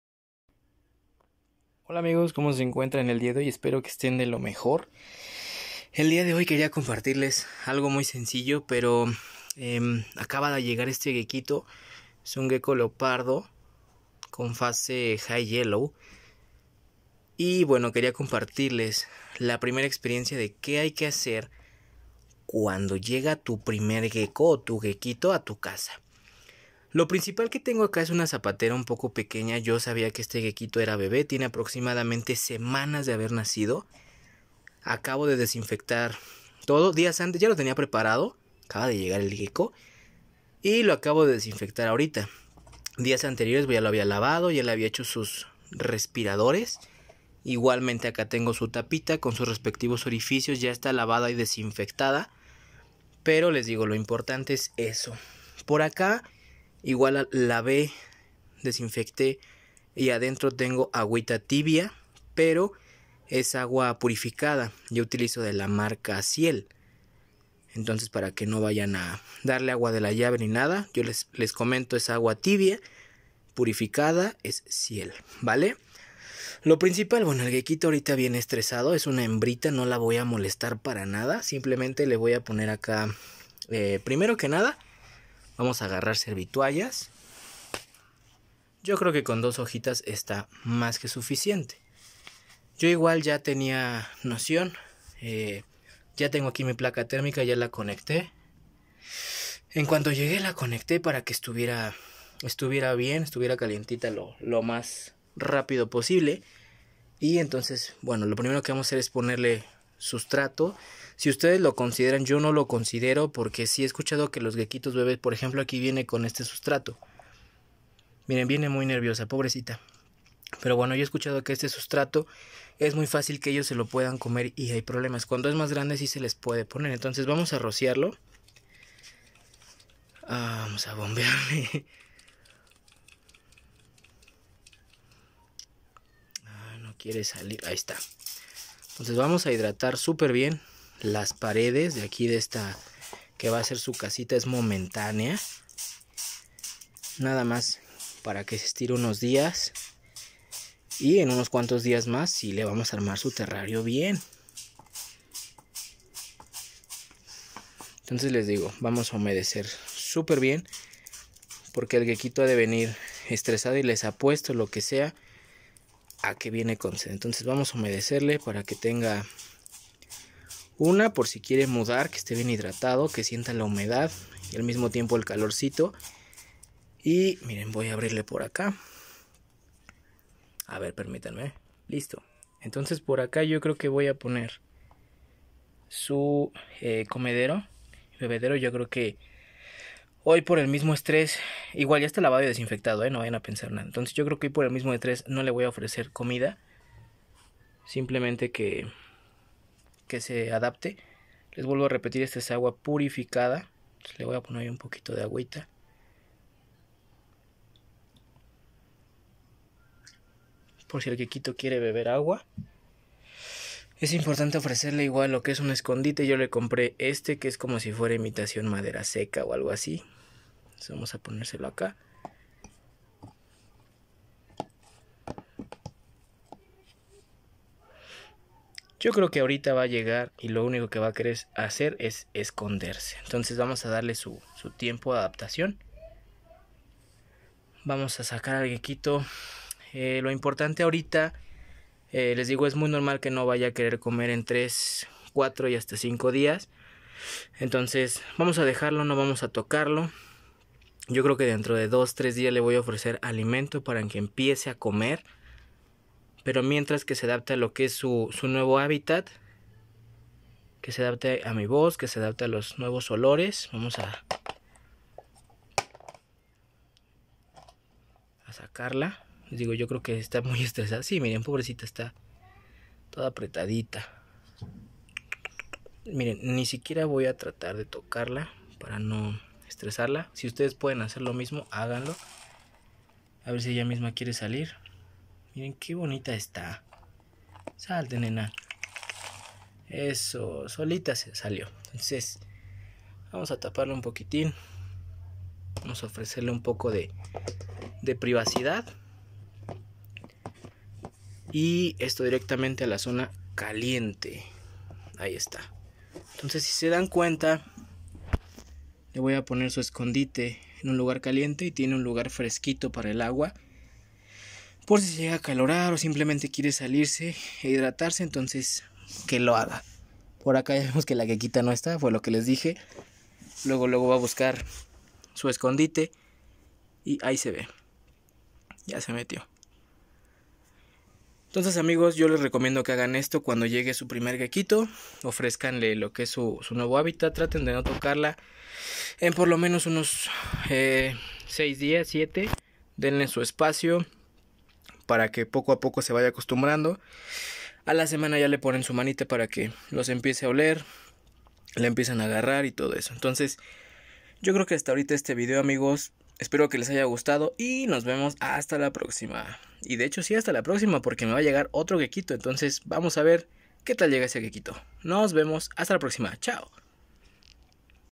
Llegó mi primer gecko. Y ahora qué hago?
Amigos. Con este video les muestro de una mabera muy sencilla como recibir a nuestro primer gecko en su nuevo hogar, ya sea un terrario, una cajita, etc.